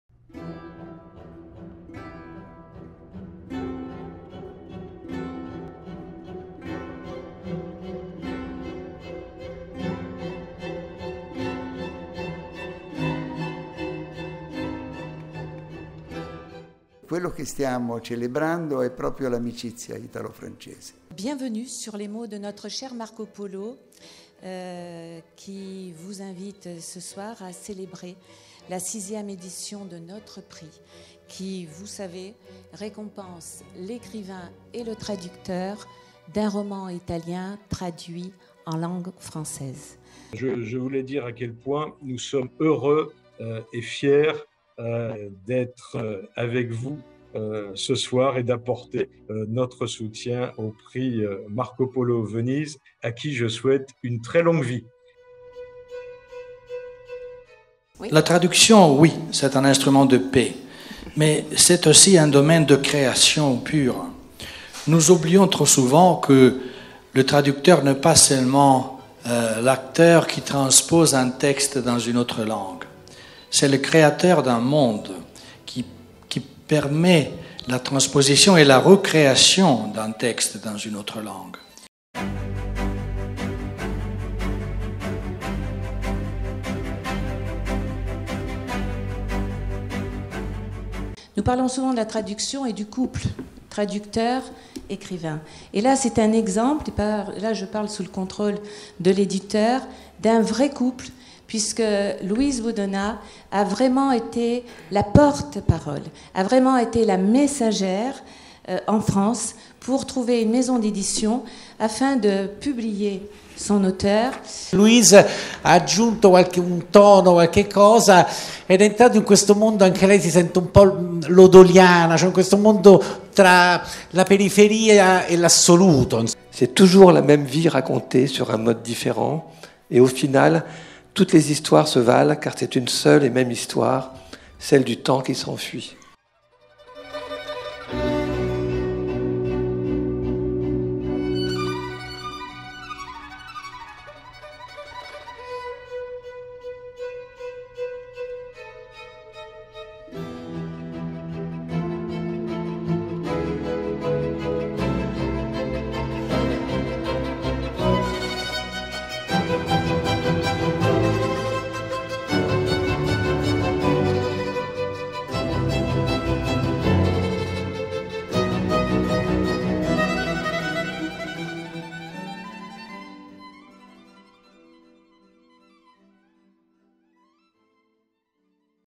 "Prix Littéraire Marco Polo Venise " 6 ème édition 2022
Quand l’histoire s’invite au présent et nous offre la richesse de notre culture et de notre civilisation. Joie de célébrer à Venise, notre écrin, notre espace de liberté, la 6 ème édition de notre « Prix Littéraire Marco Polo Venise »
Aujourd’hui notre époque est en quête de sens et nous aussi. Comment renouer avec un nouvel art de vivre dans le monde d’aujourd’hui ?
L’écrivain Paul Claudel parlait de la joie de l’homme qui s’aperçoit que le monde lui appartient parce qu’il a un sens...
Venise, ville interculturelles, nous a rappelé dans l’esprit de Marco Polo, combien les textes fondateurs de l’humanité, les manuscrits, nous permettent de répondre à cette soif qui anime notre désir de suivre le mouvement de la vie, de créer un lien entre le passé et le présent.

Table ronde sur le thème : « La traduction, la langue de la paix » avec les membres du jury et Diego Marani, Directeur de l’Institut Culturel Italien à Paris
Remise du prix aux deux lauréats : Marco Lodoli (écrivain) et Louise Boudonnat (traductrice) « Les Prières » ( P.O.L ).
Interlude avec Leili Anvar (une grande traductrice de la langue persane ) qui nous a fait voyager sur la route de la soie jusqu’en Chine pour chercher la Beauté et revenir à Venise avec l’Amour.